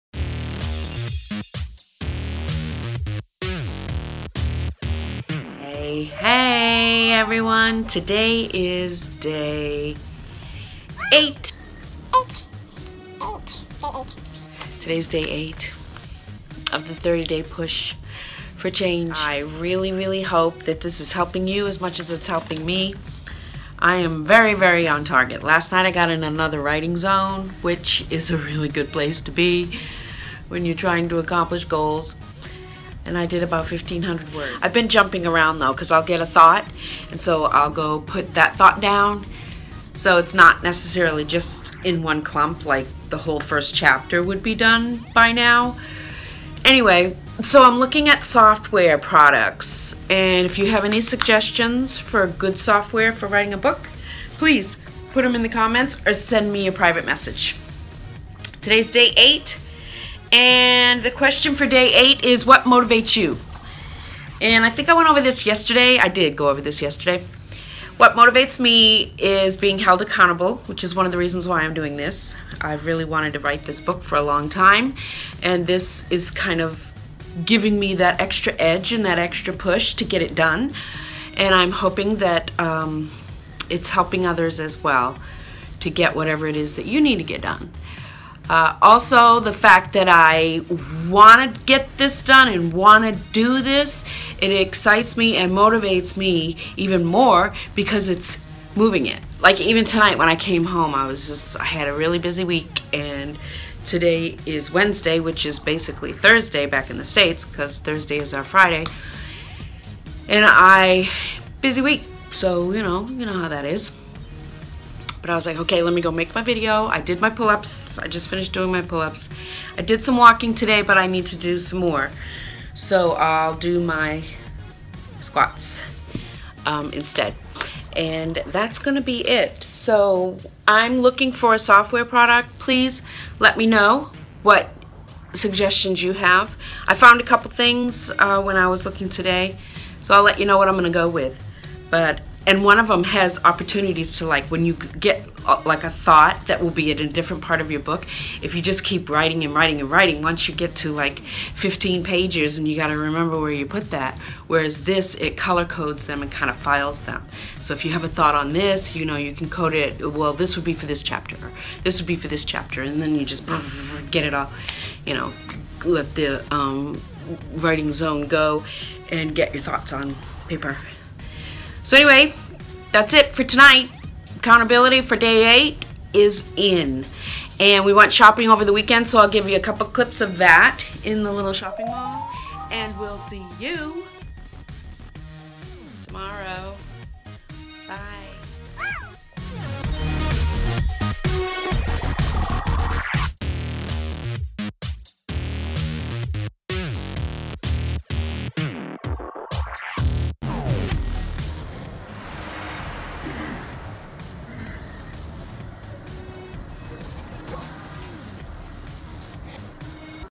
30 Day Push Forward D08
Day 08 Accountability
30-Day Daily VLOG for accountability and some more real life with Jackie on her Journey.
Thank you for pushing forward for change and making progress on your goals, we are in this together. What is your 30 Day Goal? Are you on target? One step closer for 30 days can have a major impact. Join me and let's Do This! Thanks for all your international support and wonderful comments.
Follow on Twitter: Jquee